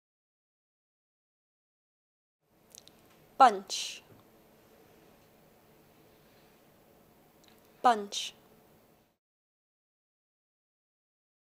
How to pronounce BUNCH in British English
This video shows you how to pronounce BUNCH in British English. Speaker has an accent from North Lanarkshire, Scotland. Collins Dictionary, the home of living English and pioneers of dictionary publishing